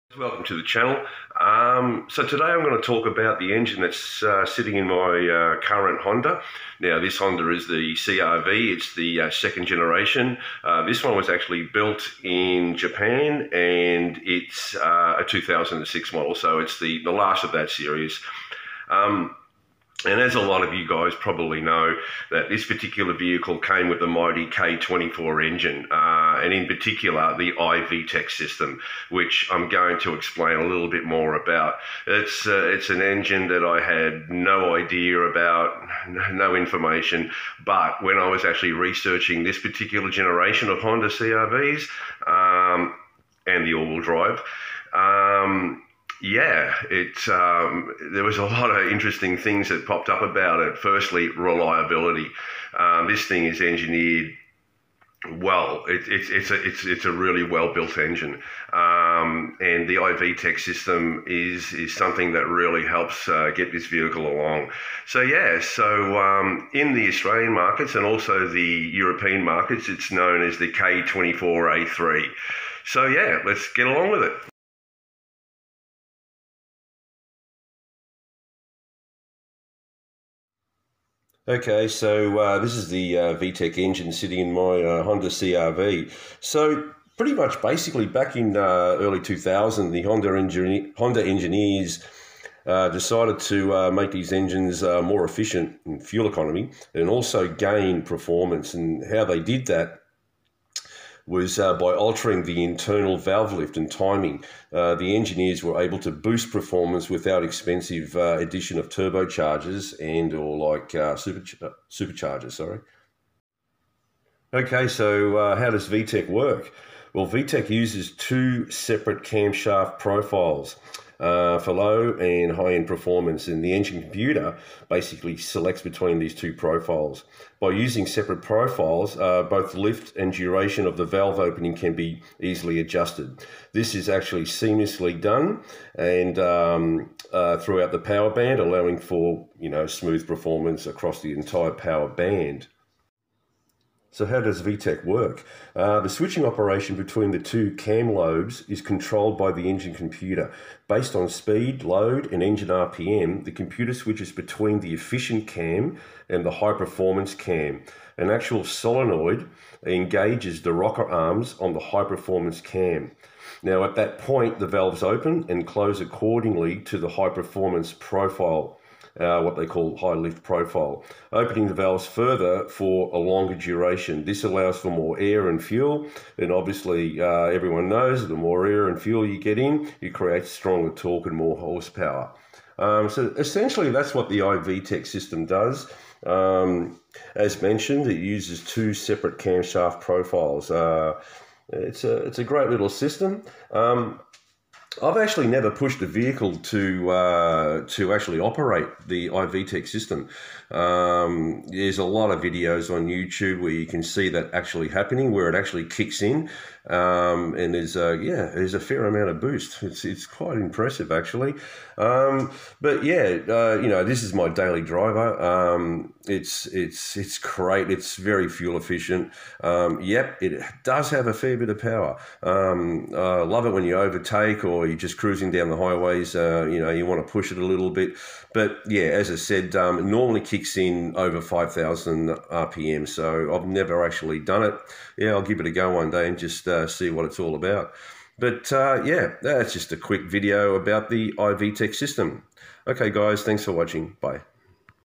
HONDA 2.4 DOHC I-VTEC ENGINE, the best Honda ever built.
The Honda k24 was introduced with the VTEC system. this uses two camshaft profiles, this creates for good fuel economy on the lower side for normal driving and high end performance when over 5000 RMP.